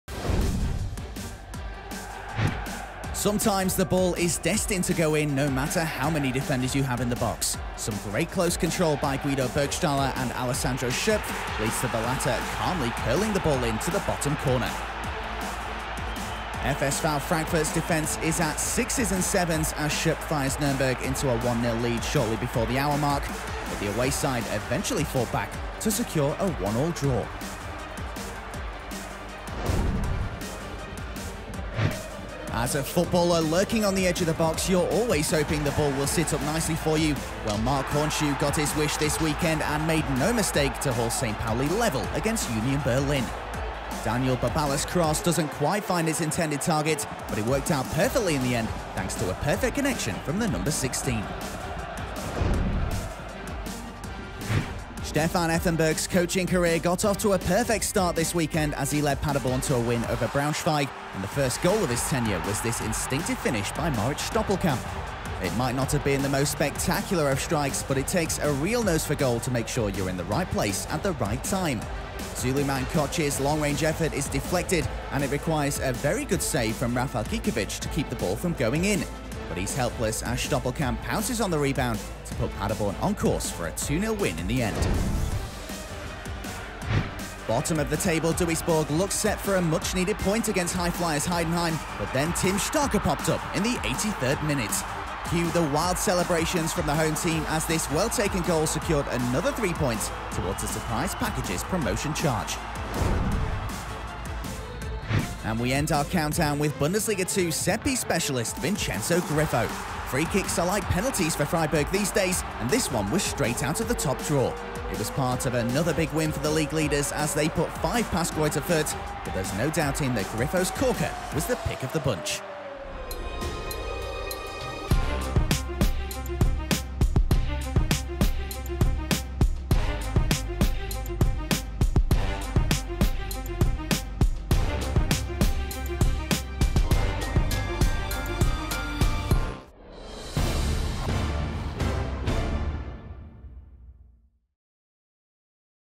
Sensational Top 5 Goals on Matchday 11
Nürnberg's Alessandro Schöpf, Union Berlin's Marc Hornschuh, Moritz Stoppelkamp from SC Paderborn 07, Heidenheim's Tim Skarke, Vincenzo Grifo from SC Freiburg, wowed with fantastic goals on matchday 11 in Bundesliga 2.